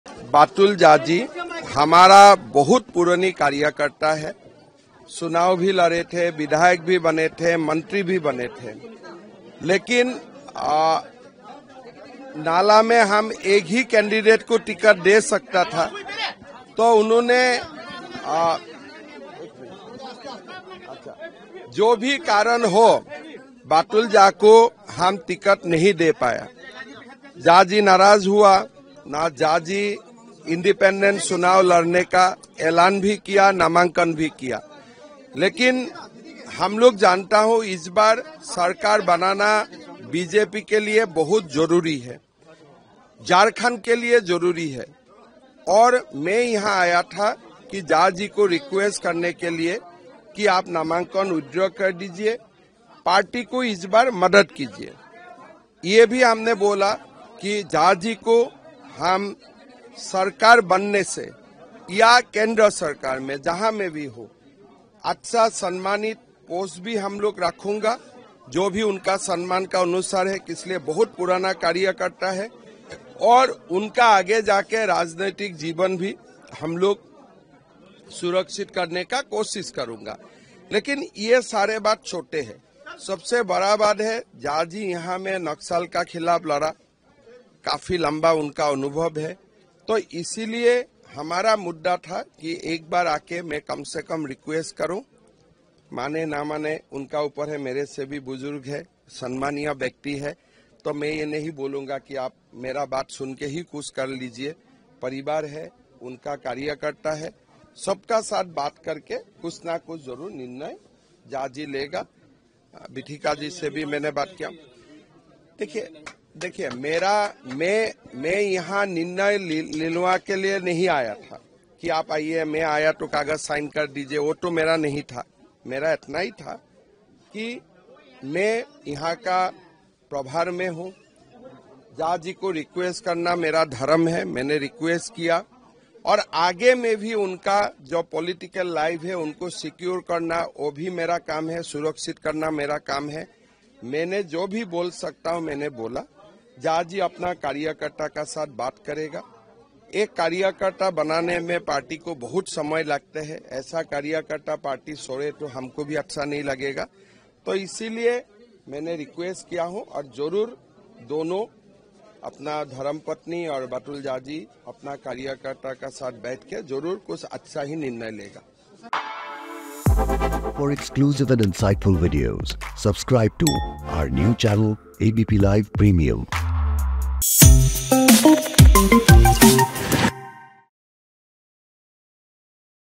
Jharkhand News: BJP नेता सत्यानंद झा के निर्दलीय नामांकन करने पर Himanta Biswa Sarmaजानिए क्या कहा
ABP News TV | भाजपा नेता सत्यानंद झा के झारखंड विधानसभा चुनाव के निर्दलिय नामांकन दाखिल करने पर असम के मुख्यमंत्री हिमंता बिस्वा सरमा ने कहा कि "हम पार्टी से एक ही व्यक्ति को टिकट दे सकते थे, हम इन्हें टिकट नहीं दे पाए इसलिए झा जी नाराज हो गए और निर्दलिय चुनाव लड़ने का एलान कर दिया। उन्होंने नामांकन भी कर दिया। इस बार भाजपा के लिए सरकार बनाना बहुत जरूरी है, झारखंड के लिए जरूरी है। मैं यहां झा जी से आग्रह करने आया था कि आप नामांकन वापस लेकर पार्टी की मदद करें। हमने ये भी कहा है कि झा को राज्य या केंद्र सरकार में अच्छा सम्मानित पोस्ट देंगे। वो बहुत पुराने कार्यकर्ता हैं... झा जी अपने कार्यकर्ता के साथ बात कर फैसला लेंगे।"